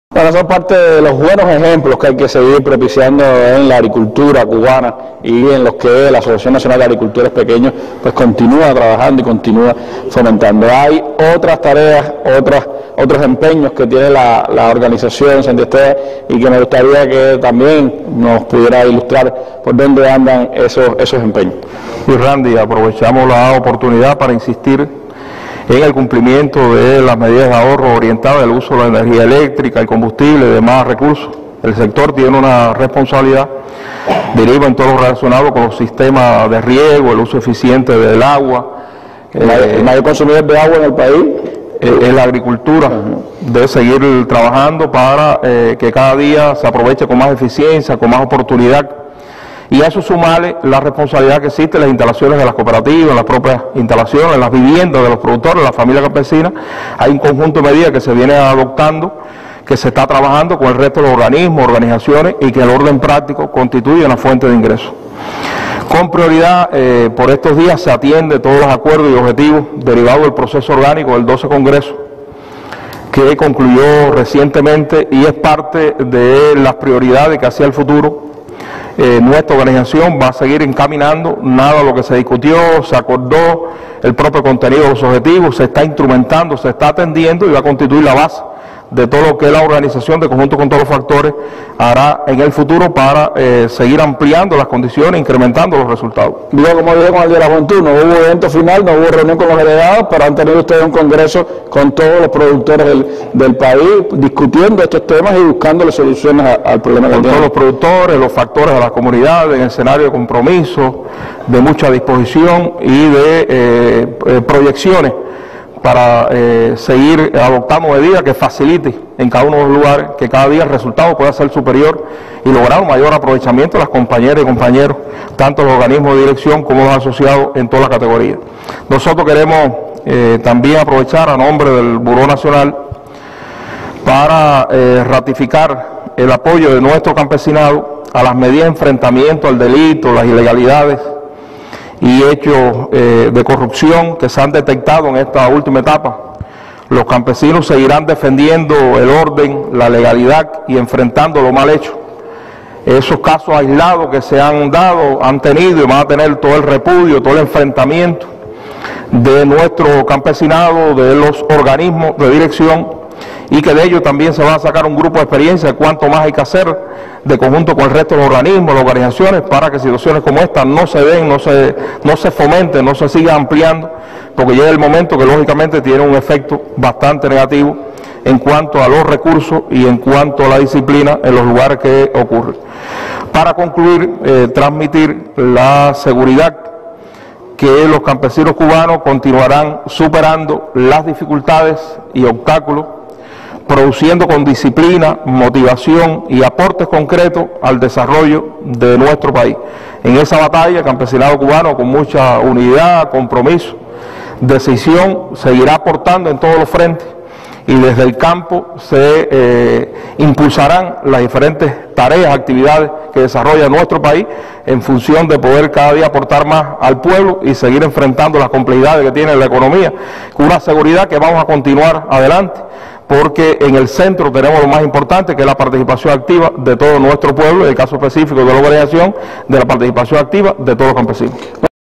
¿Qué tareas enfrenta la ANAP en Cuba en las circunstancias actuales?
¿Qué tareas enfrenta la agricultura y la ANAP en Cuba en las circunstancias actuales? ¿Cuáles son los retos de los campesinos cubanos en la producción de alimentos y en el aporte a la economía del país?